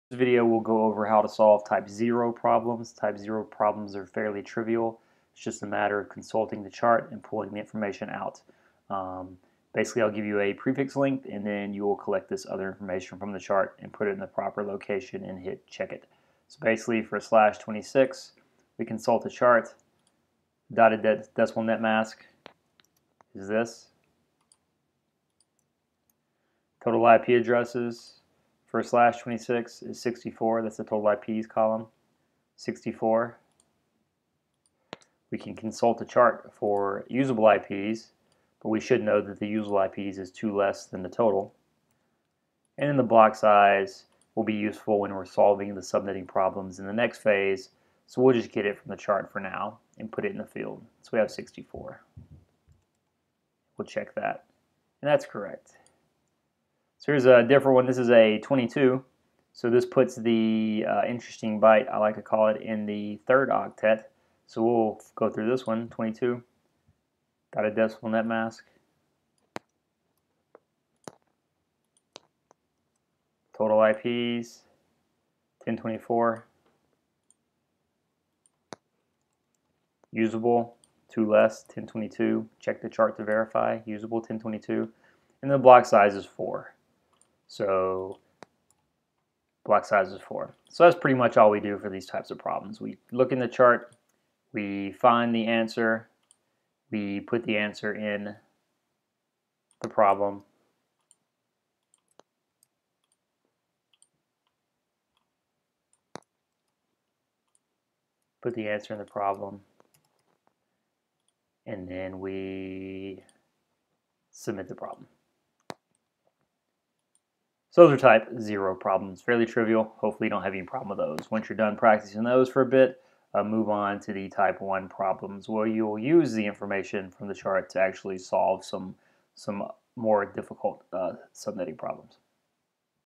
How to solve T0 problems
This video shows how to read the subnetting chart that is the basis for binary-less subnetting method demonstrated